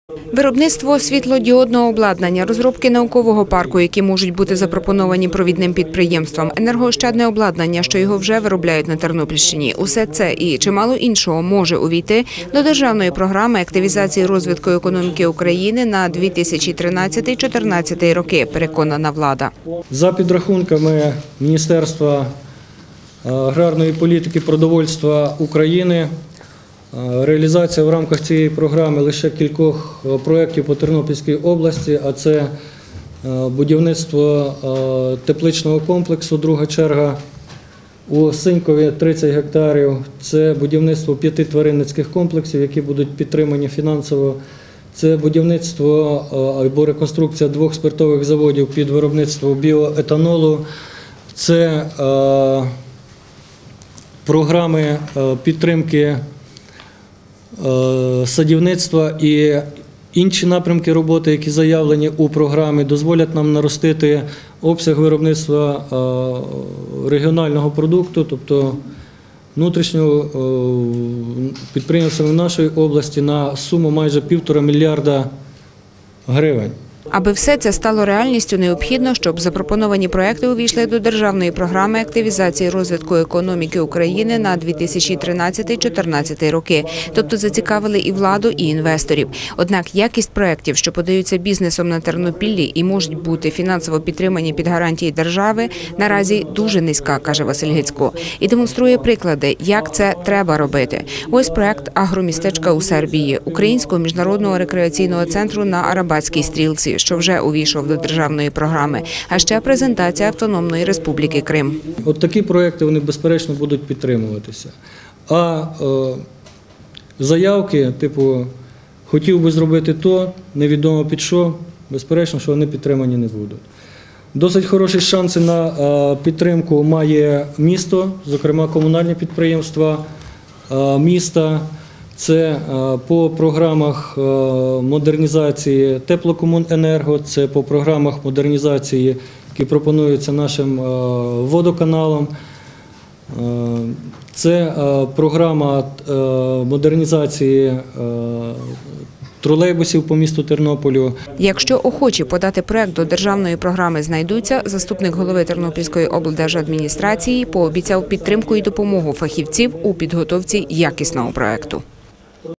Презентували програму активізації розвитку економіки
Продукції на майже півтора мільярда гривень більше, як тепер, зможе виробляти Тернопільща вже через кілька років, якщо бізнесмени та науковці напишуть проекти, що зацікавлять державу та інвесторів і увійдуть у Державну програму активізації розвитку економіки України на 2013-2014 роки. Цю програму представили керівникам управлінь облдержадміністрації і журналістам. Глобальна мета програми - зробити Україну економічно сильною державою. Є й механізм: фінансувати конкретні проекти будуть насамперед українські інвестори. Серед нових підходів і державний Банк розвитку, який планують створити в Україні. Він надаватиме реальному сектору економіки довгострокові кредити під невисокі відсотки і залучатиме кошти міжнародних інвесторів.